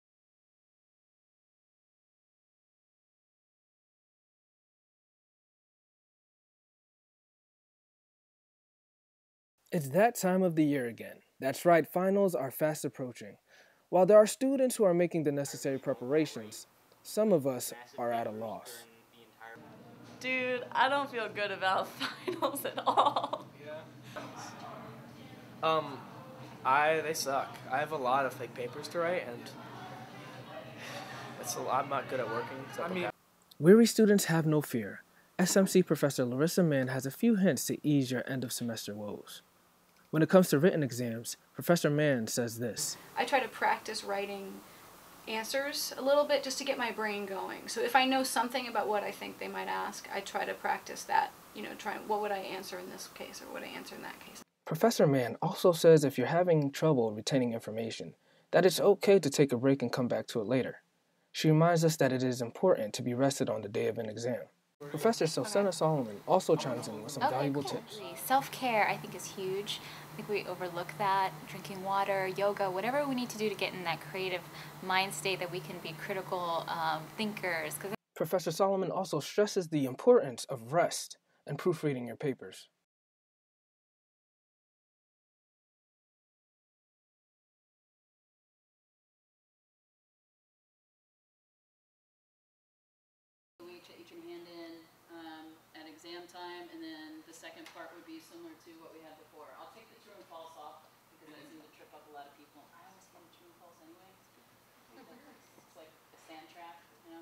Finals Tips And Tricks
Tips from professors on how to do well on your final exams - Abdul Sesay